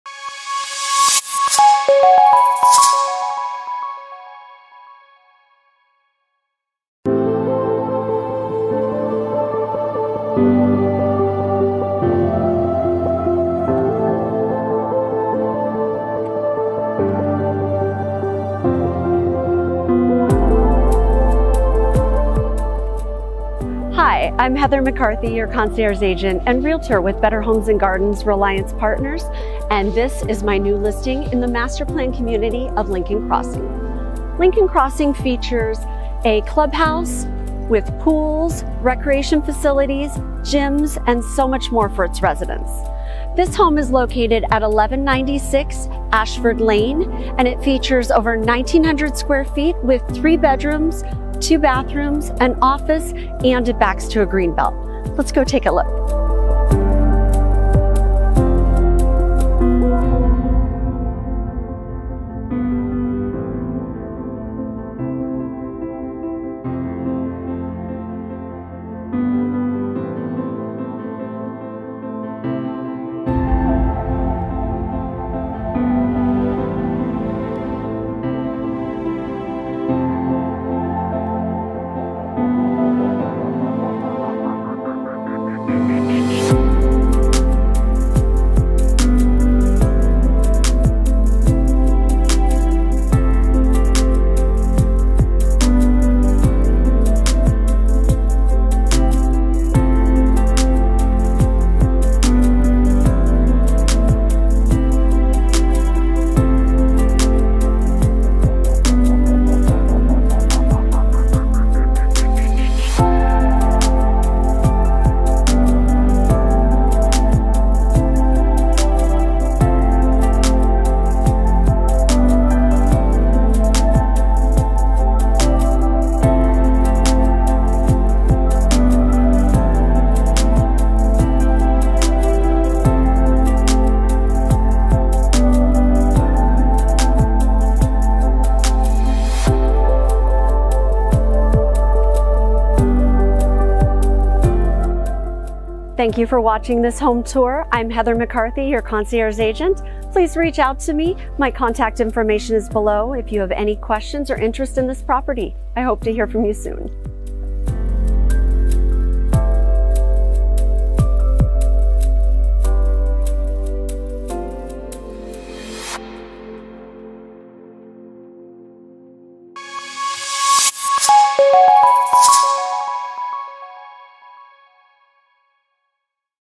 SOLD with Multiple Offers! Ashford Lane in Lincoln Crossing- In the House with Heather McCarthy
This beautiful home backed to a nature preserve greenbelt and conveniently located in the Master Planned Community of Lincoln Crossing in Lincoln,CA.

Property Description:
This 1 Story home features 3 Bedrooms plus Office, with a low maintenance yard & backs to the Nature Preserve. This 1919 sq.ft. floor plan features plank style flooring, open kitchen w/ updated lighting & appliances w/ 5 Burner Gas Range & over-range microwave, stainless steel single basin sink at a comfortable Breakfast Bar, ample counter space & a large corner pantry. The Home Office is near the front, separate from bedrooms & can be used as a flex-space. The Primary Bedroom Suite features dual bathroom sinks, soaking tub, shower & walk-in closet. The Laundry Room has additional cabinet storage & counter area. 2 Car Garage w/ Cabinetry, utility sink & security screen door. The low maintenance yard has drip irrigation, raised garden planter, greenbelt view & artificial turf. This home is located in the Master Planned Community of Lincoln Crossing, w/ walking trails, nature areas, and Resident Access to the Private Clubhouse w/ Resort-style amenities. Residents have 3 huge outdoor pools, an indoor lap pool, Kids' Splash Pad & Shade Structures. Enjoy Weekly Workout classes or hit the Gym with Weights & Cardio Equipment. Meet your neighbors & enjoy monthly events w/ live music & activities for the kids. High Speed Internet is also Included with the HOA.

This home sold above List Price with 3 Offers!
Reach out to fin out what makes the properties that I list SELL for MORE $$$!
Heather McCarthy
DRE#02049759